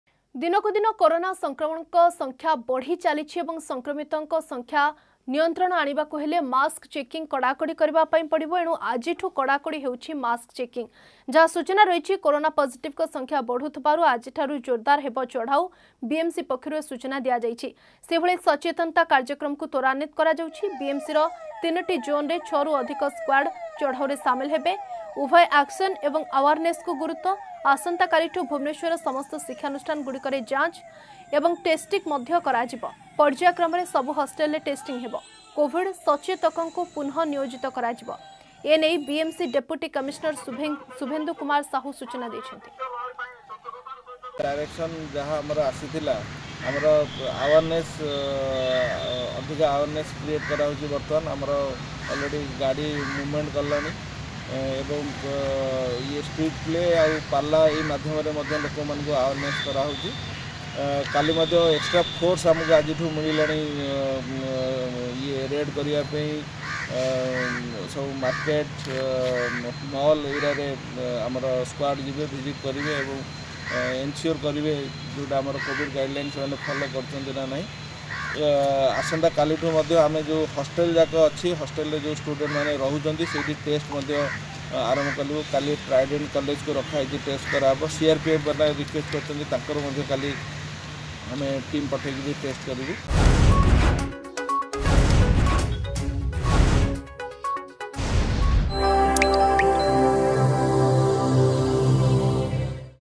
କାୟା ବିସ୍ତାର କରୁଛି କରୋନା
ରାଜ୍ୟରେ କଡାକଡି ହେଉଛି ମାସ୍କ ଚେକିଂ...